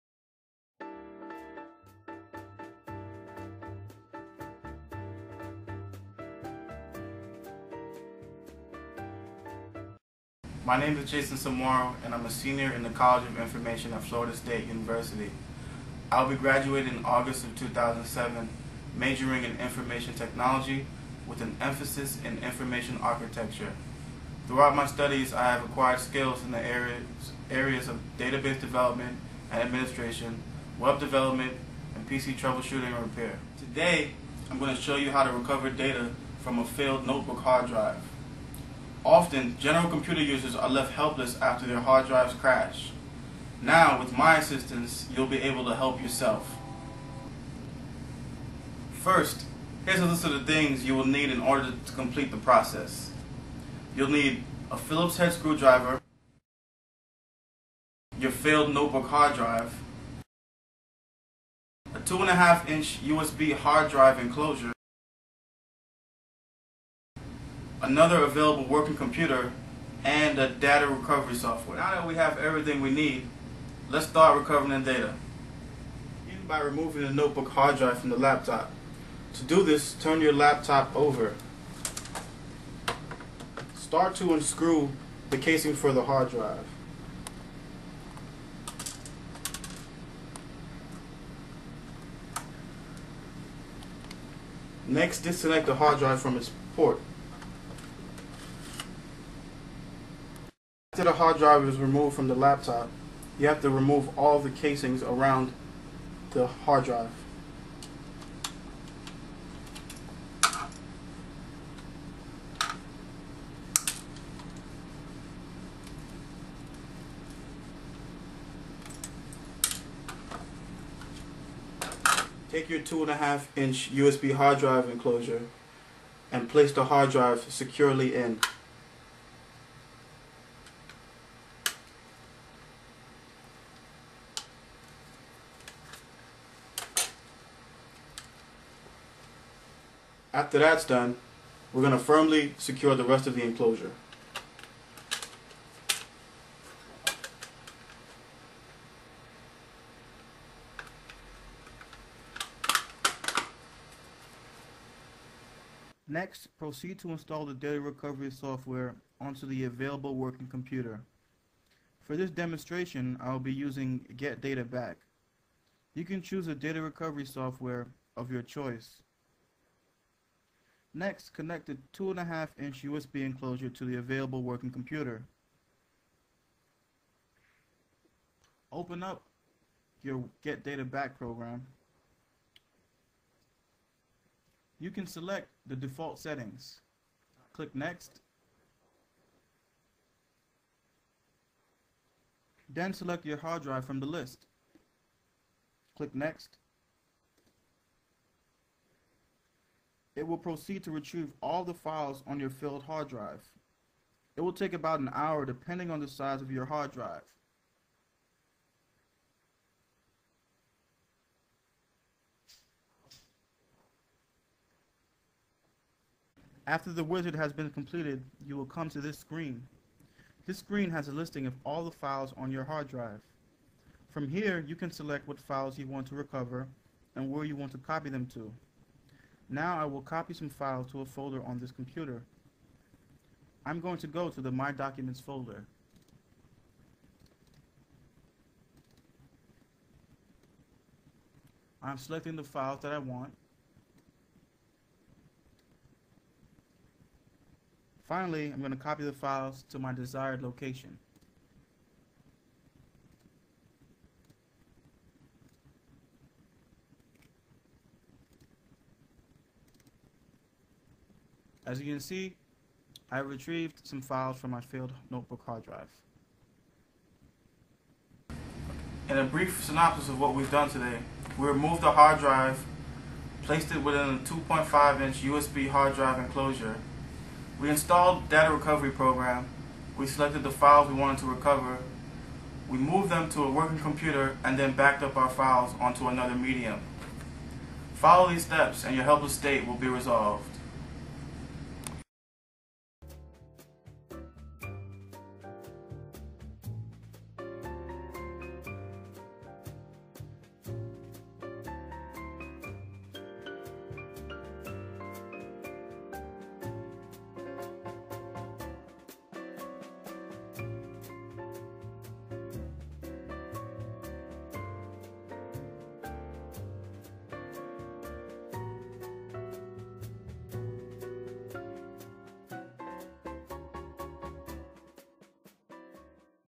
How to recover data from a failed notebook hard drive.
A step by step tutorial on how to recover data from a failed notebook hard drive.